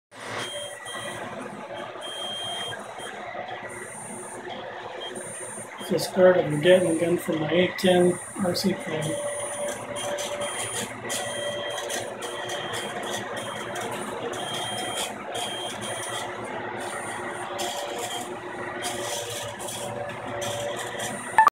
Monoprice Mini Select A-10 Gatling Gun Start Print
Showing the start of my 3D print I'm making for my A-10 RC plane